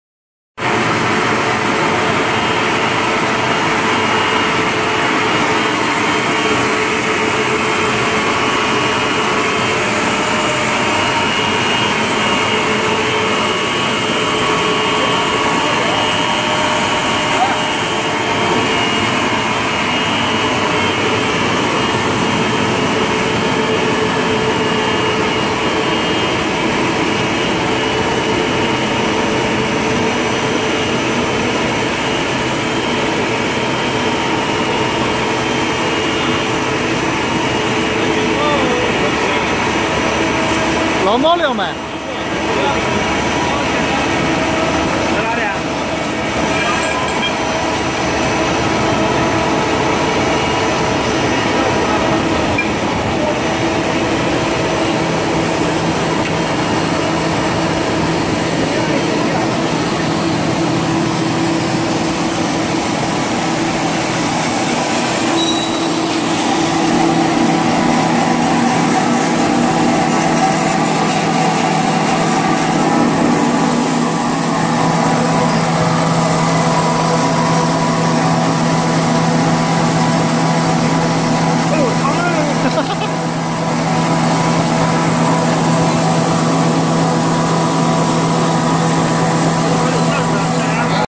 KOOEN SJ100-100 Double stage PE film granulating line (with force feeder)
This line(SJ100-100 Double stage PE film granulating line ) is from Zhangjiagang KOOEN Machinery CO., Ltd (China), video taken on year of 2014 (May), machine already exported to Russian.
Our Company information as below:
Company name: Zhangjiagang KOOEN Machinery Co.,ltd
Contactor: Mr. Lu yaping (General manager)